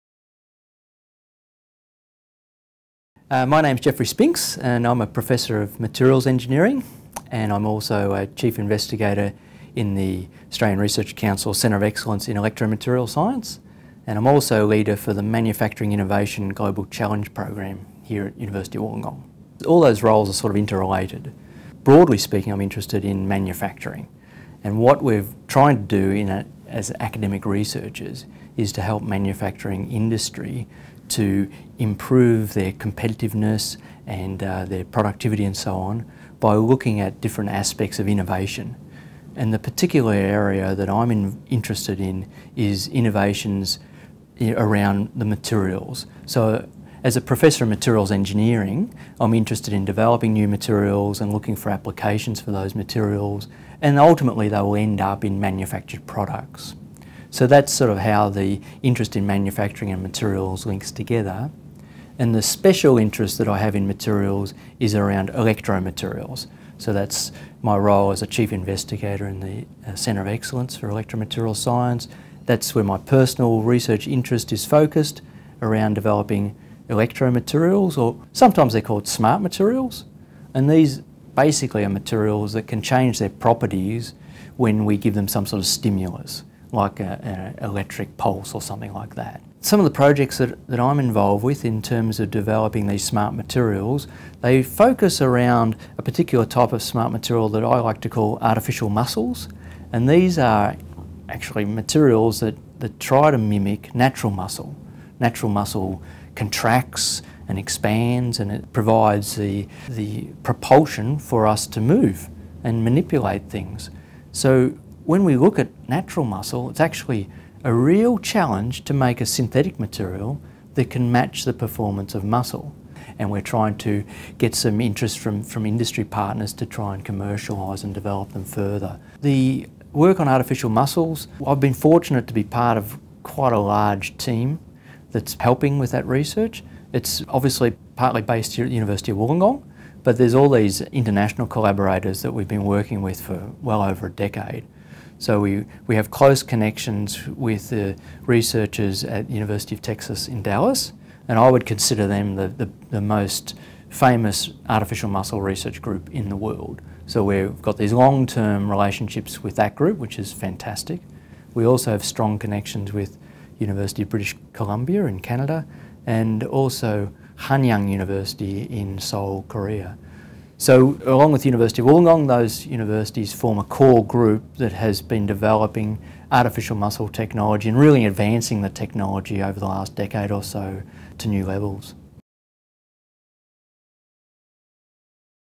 40 Years of Research Impact - Geoff Spinks
In 2014, the announcement in the prestigious journal Science that UOW researchers had used ordinary, inexpensive polymer fibres, like fishing line and sewing thread, to make artificial muscles, generated great interest around the world.